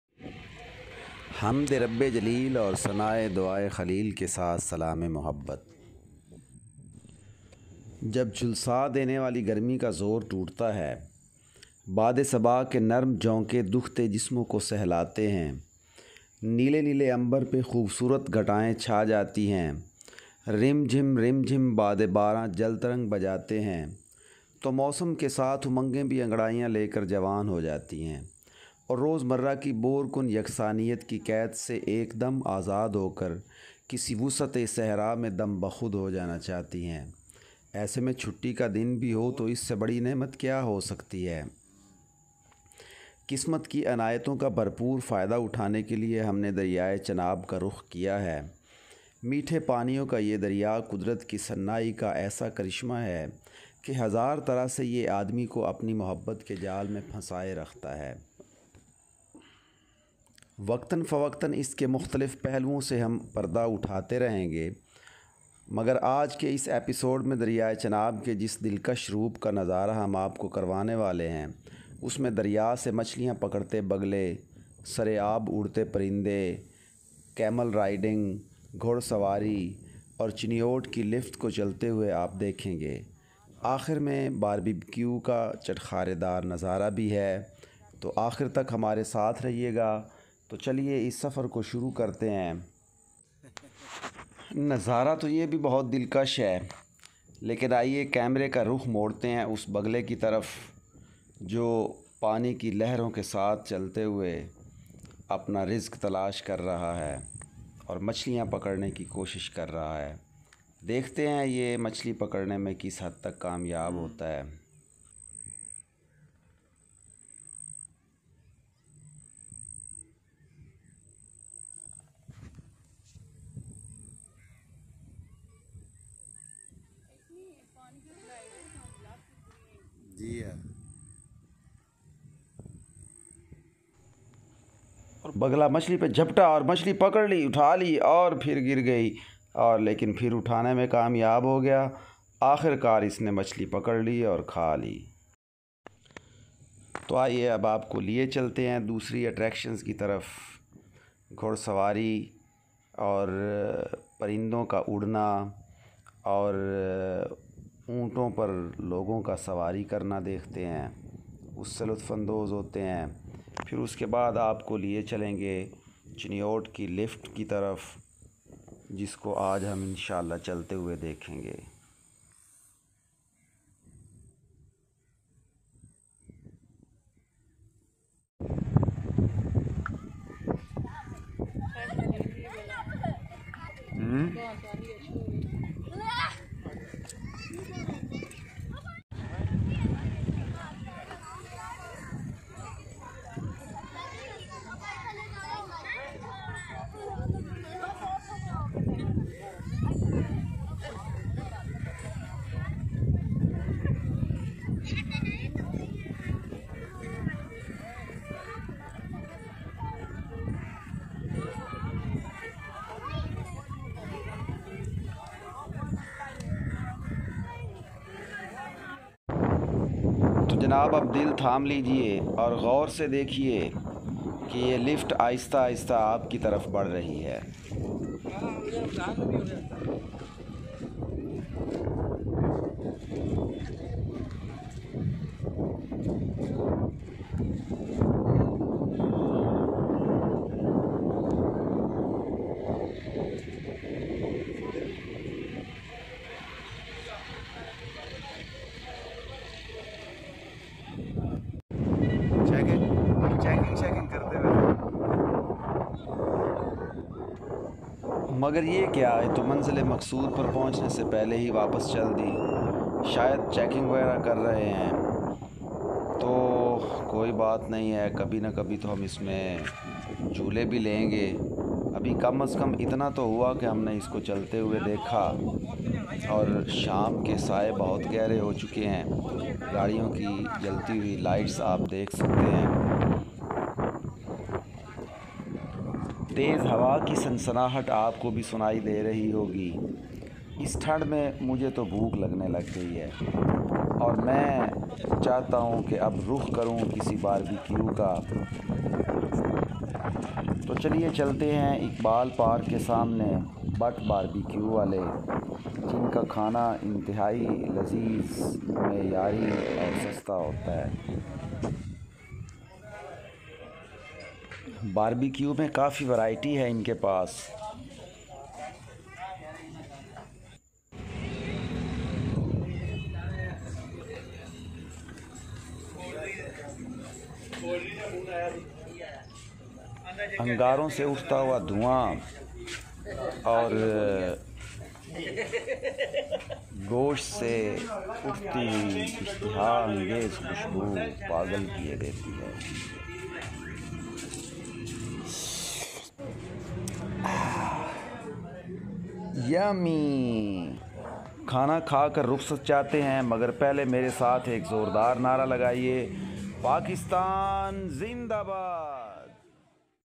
A picnic party | The Chenab River | The Chiniot cable lift | BBQ
In a picnic party, with bbq, on the Chenab River Chiniot, we saw, the cable car lift was running. The funcar channel traveled to the Chenab River to make a vlog or travelogue on seagulls catching fish, camel ride, horse ride, cable car lift and at last to enjoy a delicious bbq. We were able to enjoy all these attractions and now these are presented to the precious viewers.